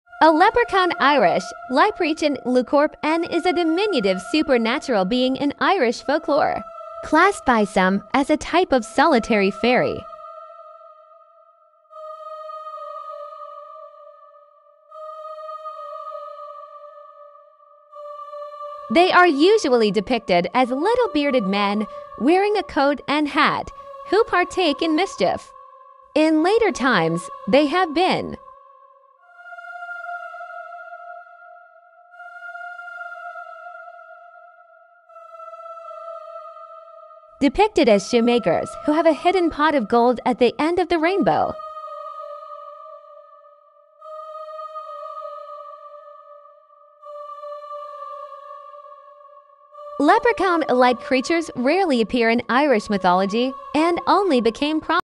leprechaun Irish leipreachán luchorpan mythology creatures
A leprechaun (Irish: leipreachán/luchorpán) is a diminutive supernatural being in Irish folklore, classed by some as a type of solitary fairy. They are usually depicted as little bearded men, wearing a coat and hat, who partake in mischief. In later times, they have been depicted as shoe-makers who have a hidden pot of gold at the end of the rainbow.

A modern depiction of a leprechaun of the type popularised in the 20th century

Leprechaun-like creatures rarely appear in Irish mythology and only became prominent in later folklore.